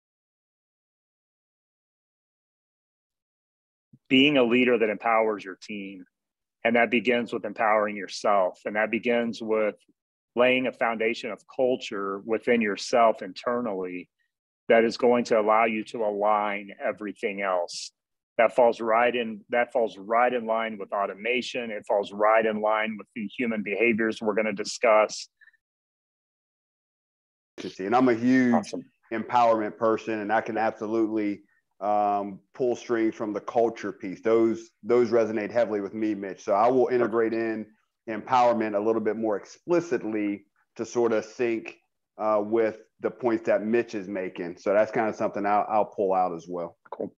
Leading Starts with a Personal Sense of Culture
Thanks for joining us in this ‘shorts’ playlist as we take a look into the content we will present at the Digital Vision Small Business Leadership Conference, February 2022. See link below for registration.

Join Mitch Gray, Eddie White, and Tyrell Watson as we discuss leadership, empowerment, and technology as tools for successful business outcomes.